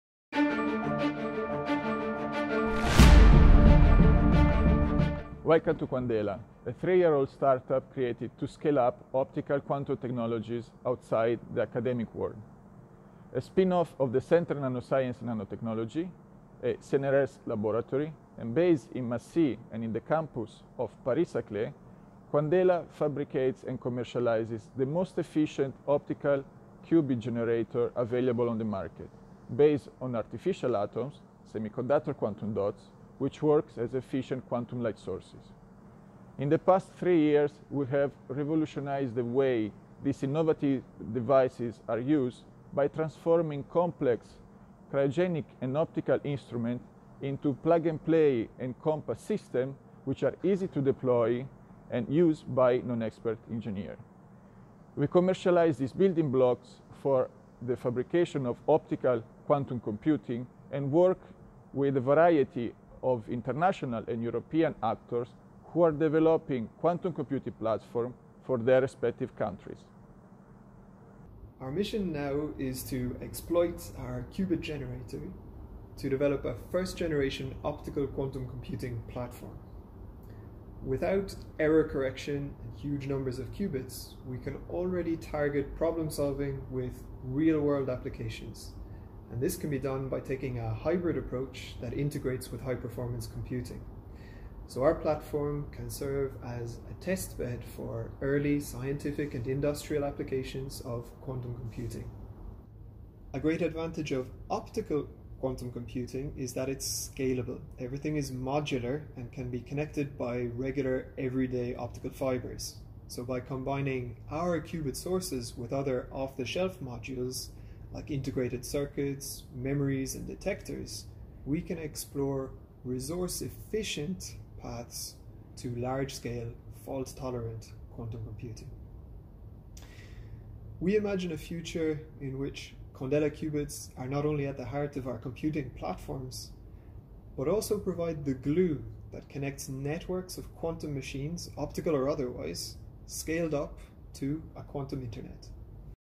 Quandela presentation 2020
Founded in 2017, Quandela is a technology company based in Massy and on the Paris-Saclay campus in France. We develop the world's most efficient photonic quantum bit emitters and the optical quantum computers based on these emitters.